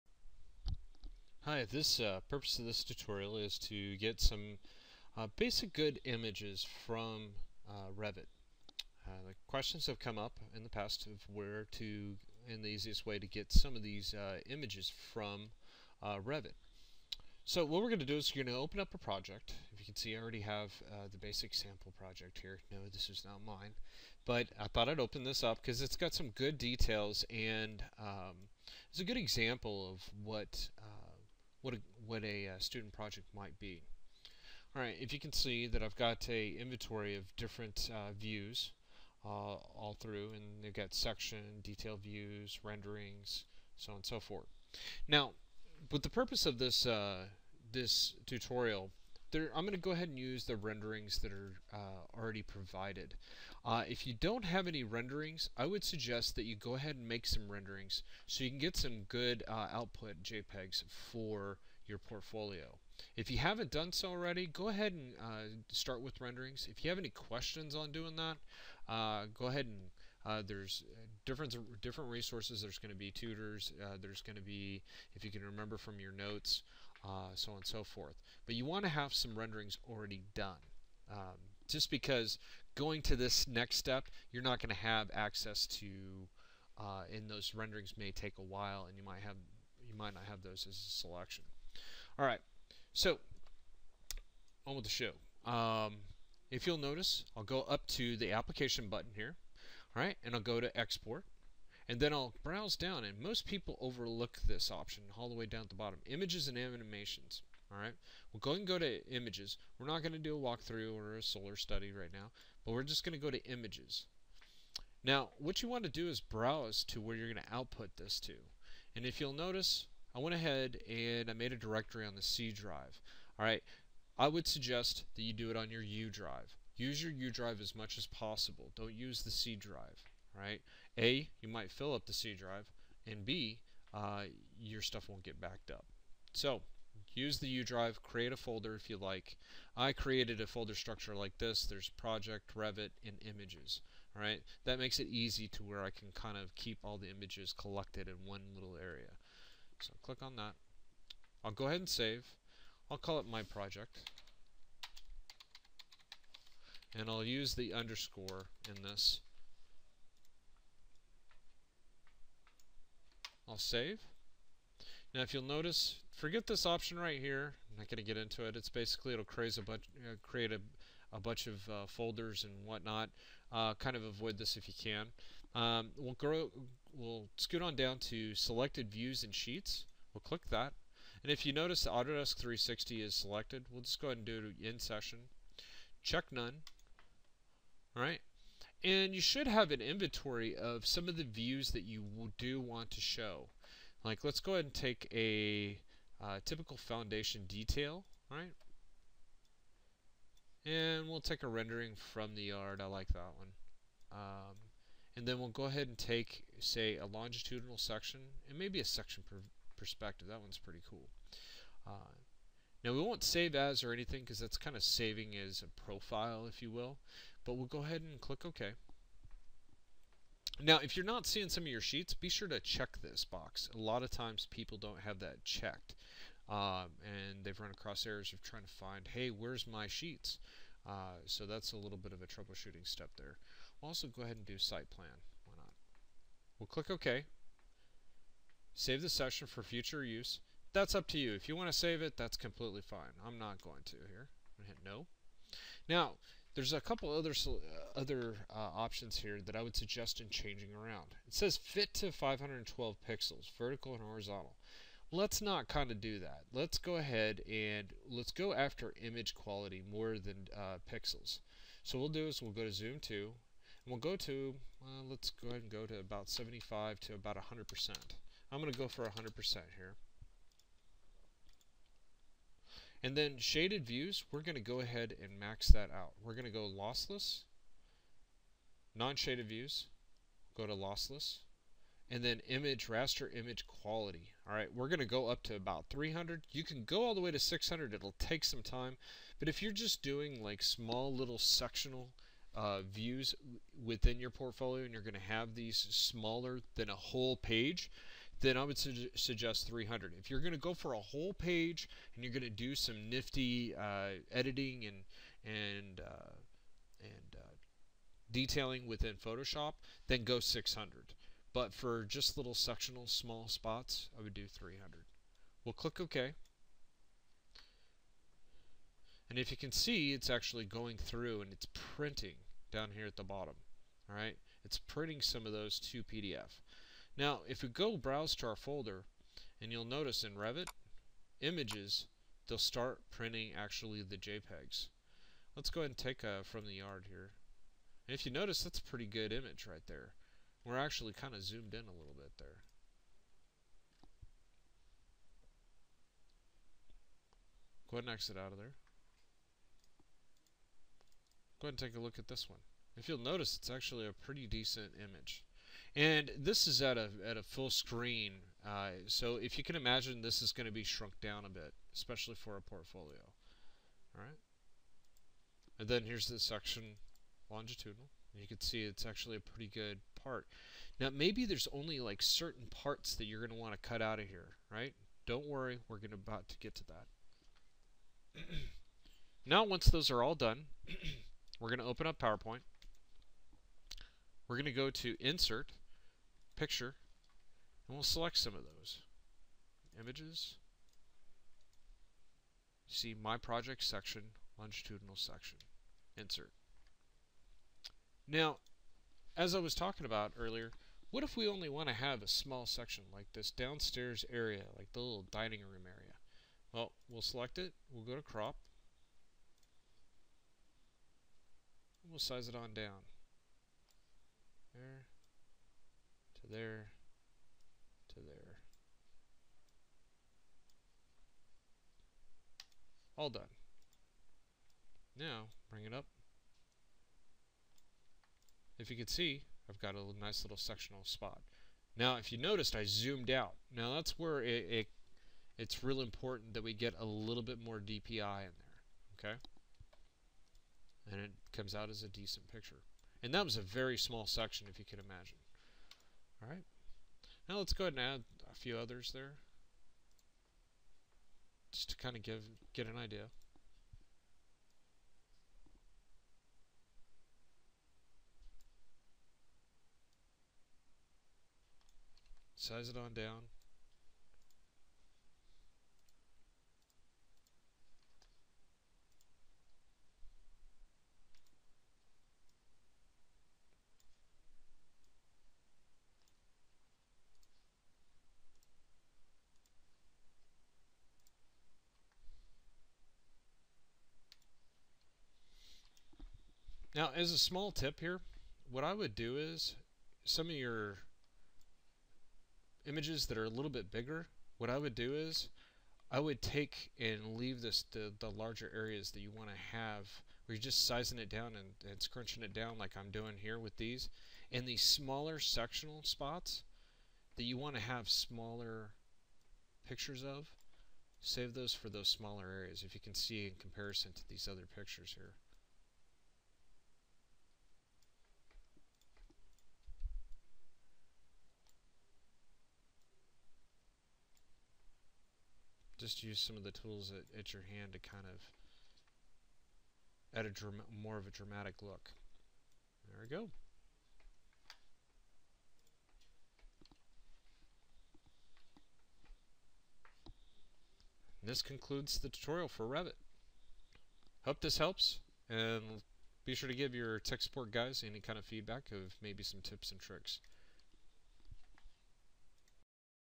Revit - High Resolution Images for Portfolio
Steps in capturing high resolution images from Revit for use in your portfolio. Video by Nathan Tobin.